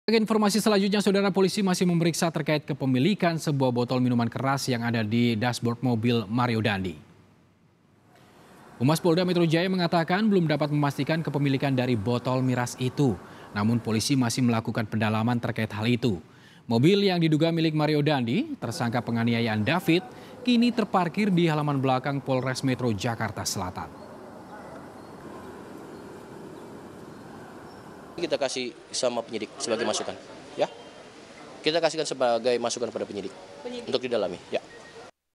Polisi Dalami Kepemilikan Botol Miras di Mobil Rubicon yang Dikendarai Mario Dandy
JAKARTA, KOMPAS.TV - Polisi masih memeriksa terkait kepemilikan sebuah botol minuman keras yang ada di dashboard mobil Rubicon yang dikendarai Mario Dandy.

Mobil Rubicon bernopol B 120 DEN yang dikendarai tersangka penganiayaan Mario Dandy Satriyo terlihat ada botol minuman keras di dalamnya.

Botol tersebut tampak tersimpan di dashboard tengah mobil dan masih tersegel.

Humas Polda Metro Jaya mengatakan, belum dapat memastikan kepemilikan dari botol miras itu.

Namun polisi masih melakukan pendalaman terkait hal itu.

Mobil yang diduga milik Mario Dandy, tersangka penganiayaan David, kini terparkir di halaman belakang Polres Metro Jakarta Selatan.